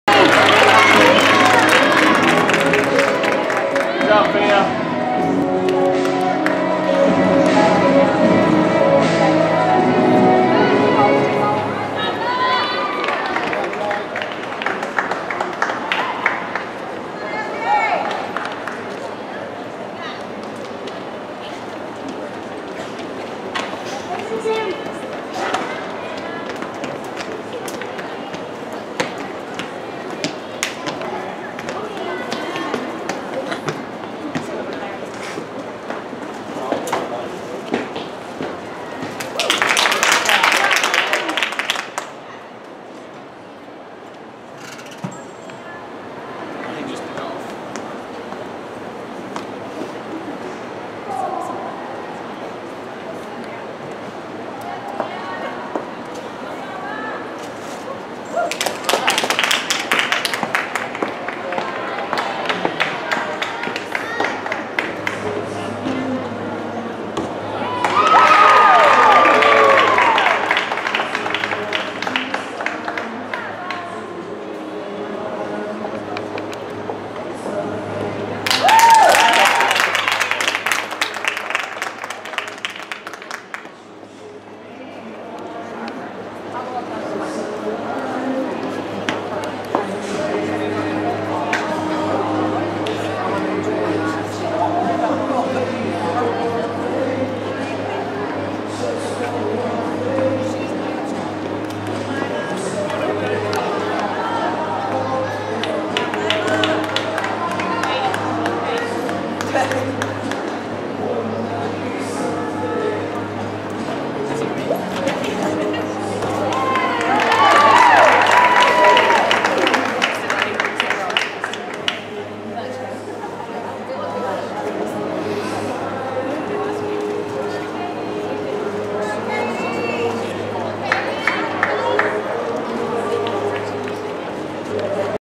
Cami Weaver Gymnastics Beam Routine 2016 Elevate the stage Birmingham
9.40 Score